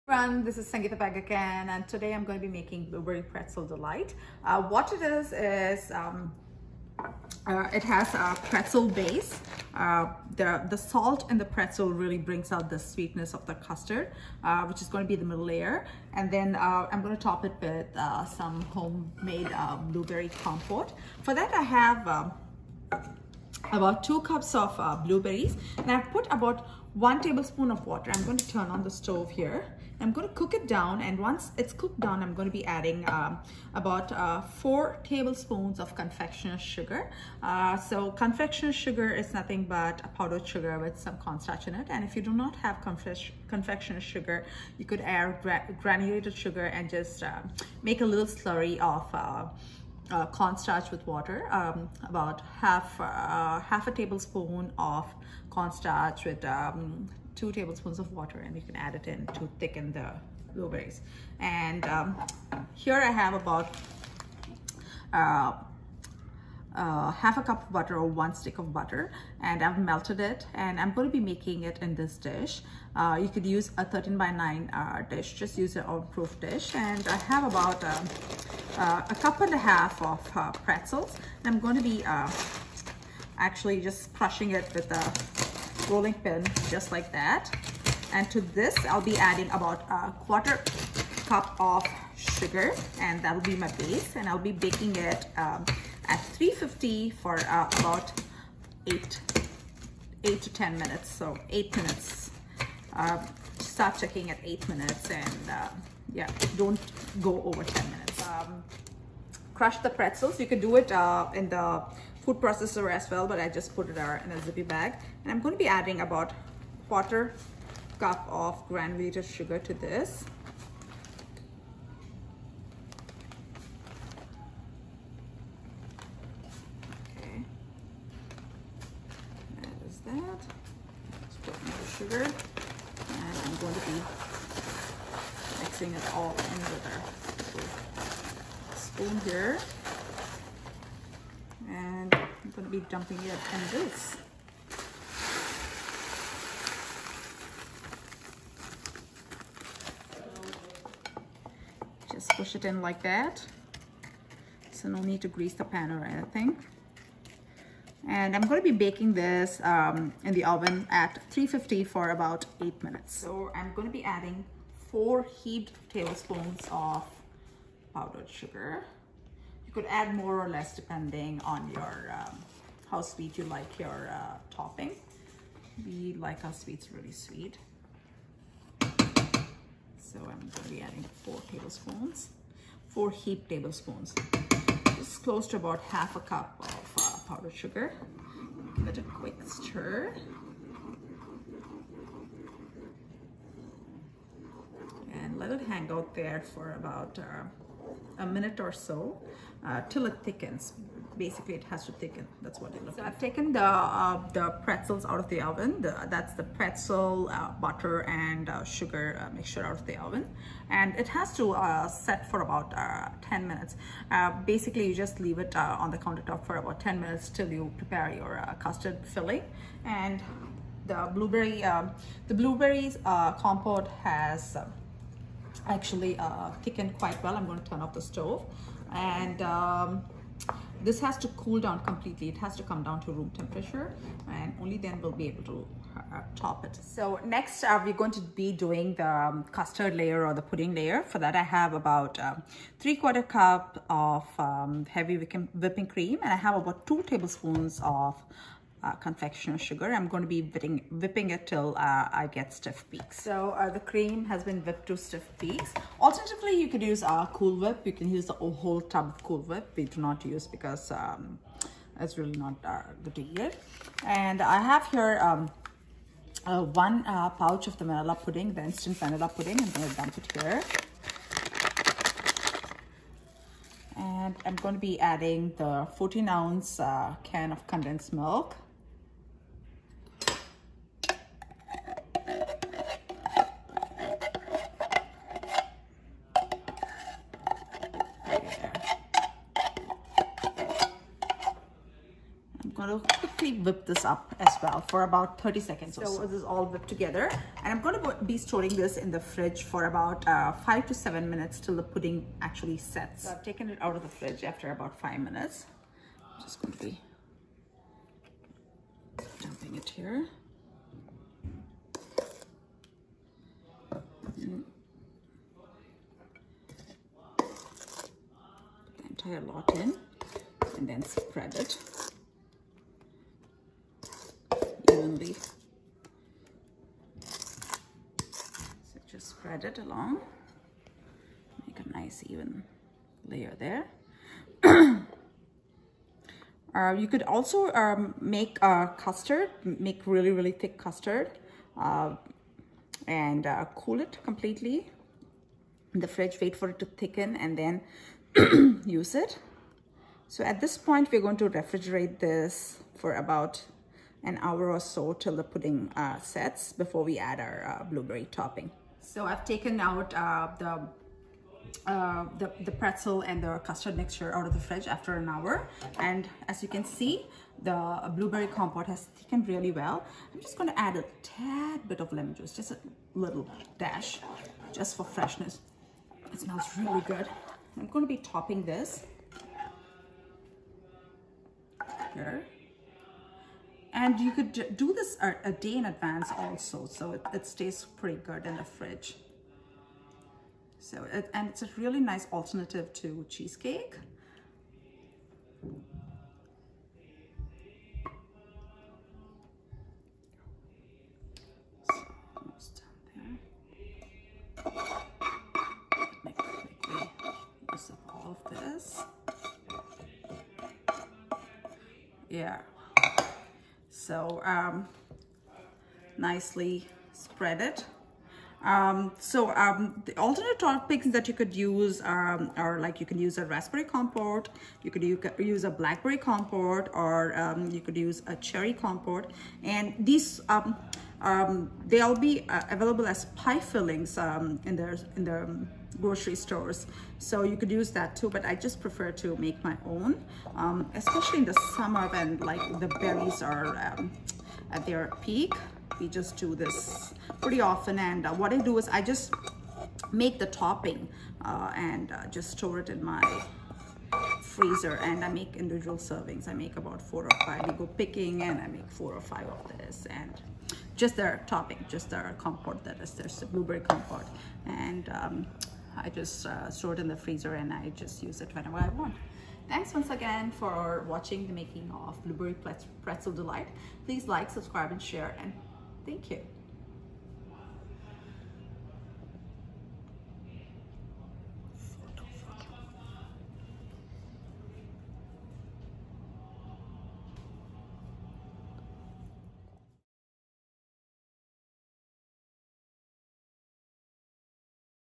Blueberry pretzel delight by Sangeetha!
A very simple dessert that can be put together without too much of a fuss!  Make this for your next party and simply dazzle your guests.

Ingredients: Pretzels, Butter, sugar, condensed milk, instant vanilla pudding mix, whipping cream, blueberries, powdered sugar.  Cool whip can be used instead of whipping cream (substitute a container of cool whip for the whipping cream).  Blueberry pie filling (store bought) can be used instead of the compote that is homemade.  Alternative toppings are cherry compote and raspberry compote.